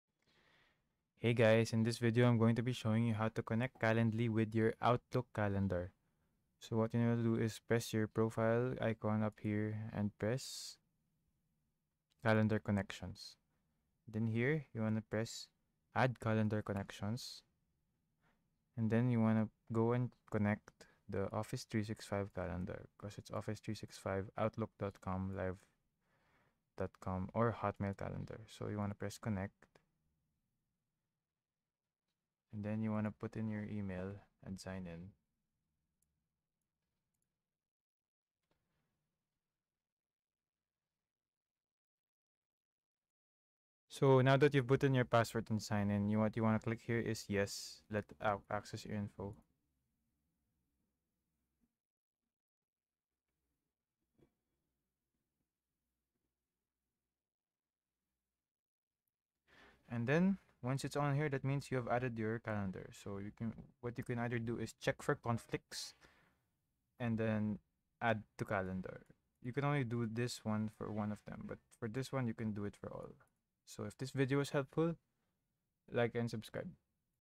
How To Connect Calendly To Outlook Calendar (2023)
If you are looking for a video about how to connect calendly to outlook calendar, here it is!
In this video I will show you how to connect calendly to outlook calendar. Be sure to watch the video to the very end.
You will learn how to connect calendly to outlook calendar in this video.